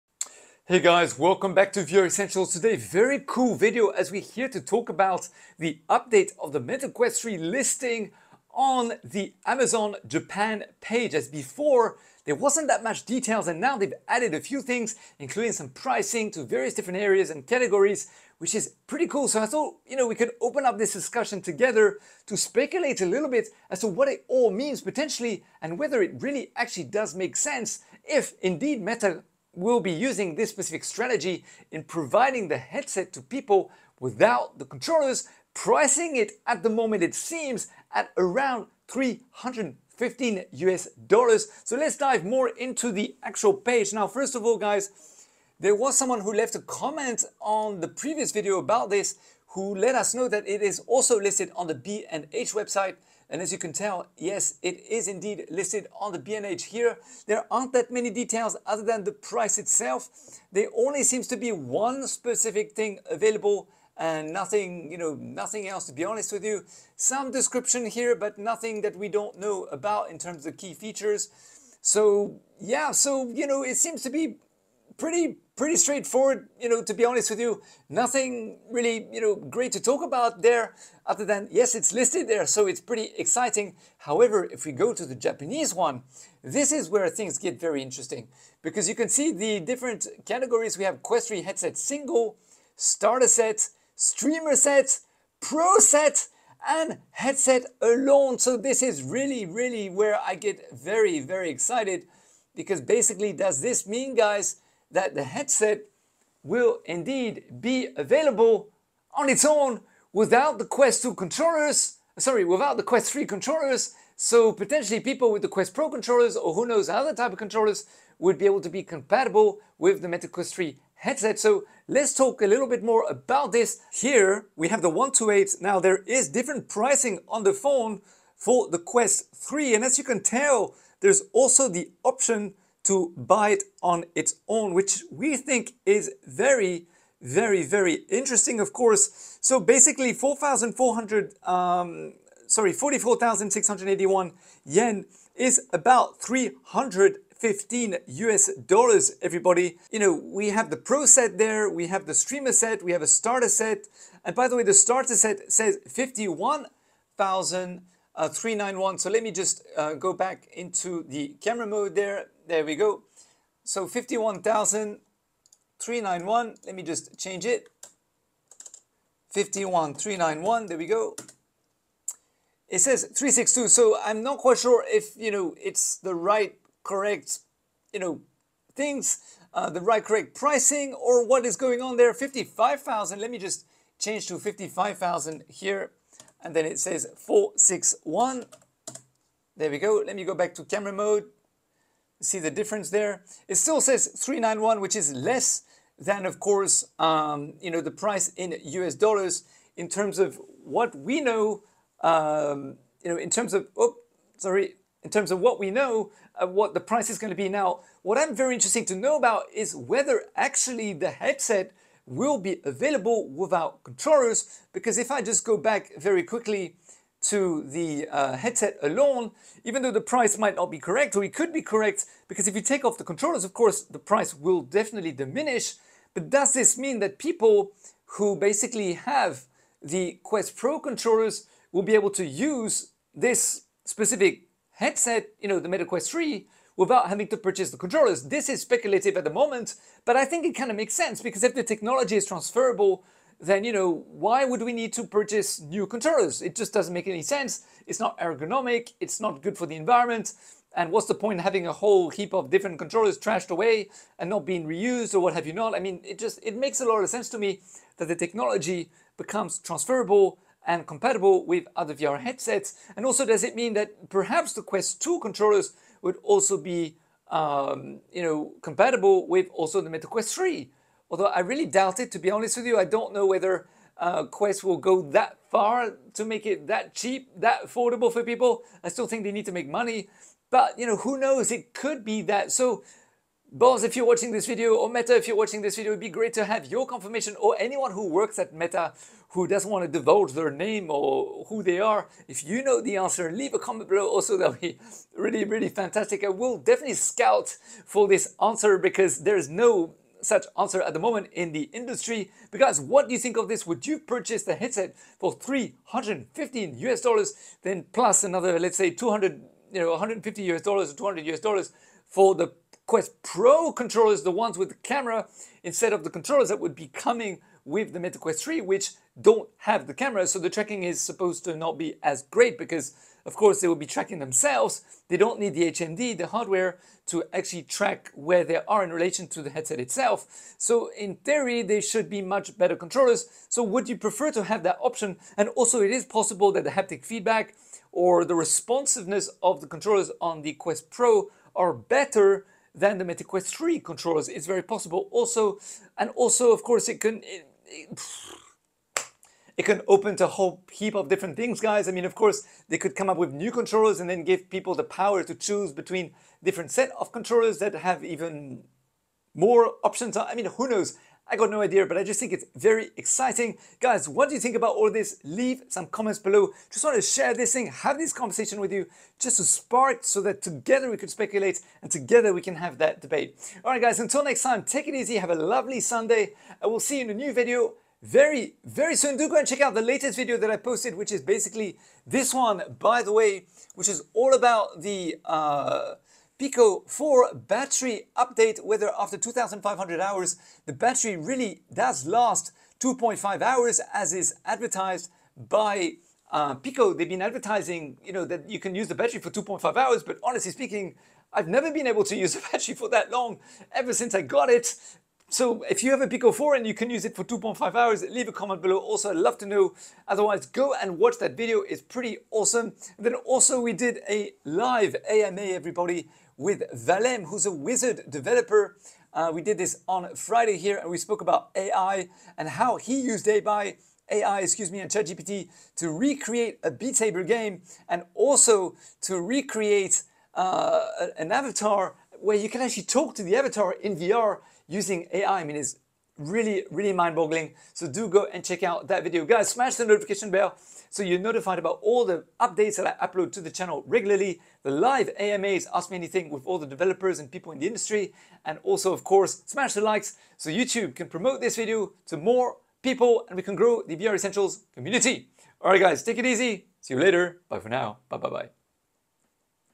META QUEST 3 FROM $315 ON ITS OWN WITHOUT CONTROLLERS ON AMAZON!
Exciting news for VR enthusiasts! The Meta Quest 3, the highly anticipated successor to the Quest 2, is finally making waves in the tech world. With Meta and Apple locked in an intense mixed reality battle, the Quest 3 aims to democratize VR technology, offering a more affordable option compared to its rival.

According to leaked specifications, the Quest 3 boasts significant improvements over its predecessor. With a thinner and lighter design, a Qualcomm Snapdragon XR2 Gen 2 processor, and a higher per-eye resolution of 2064 x 2208, this headset promises a more powerful and immersive VR experience.

One noteworthy feature of the Meta Quest 3 is that it will be a standalone VR headset, eliminating the need for tethering to a computer or console. This will provide users with more freedom of movement and accessibility.

While Meta has not yet revealed all the details, some information has surfaced regarding the controllers. It remains unclear whether the Quest 3 will be available without controllers, like the Quest 2, or if users will have the option to buy different types of controllers. Additionally, it is speculated that existing Quest 2 or Quest Pro owners will be able to use their previous controllers with the Quest 3.

As for the pricing, it's believed that the Meta Quest 3 will retail for $499, marking a $100 price increase from the Quest 2. Despite the increase, this competitive price point ensures that the Quest 3 remains accessible to those wanting to delve into the world of VR.

The release date for the Quest 3 is expected to be in Fall 2023, with an anticipated launch in October or November, just in time for the holiday season.

Overall, the Meta Quest 3 is shaping up to be a top-notch VR headset, offering excellent wireless VR experiences with its Pancake lenses and enhanced visual fidelity. For those who have been patiently waiting, it's definitely worth holding out for the official announcement from Meta.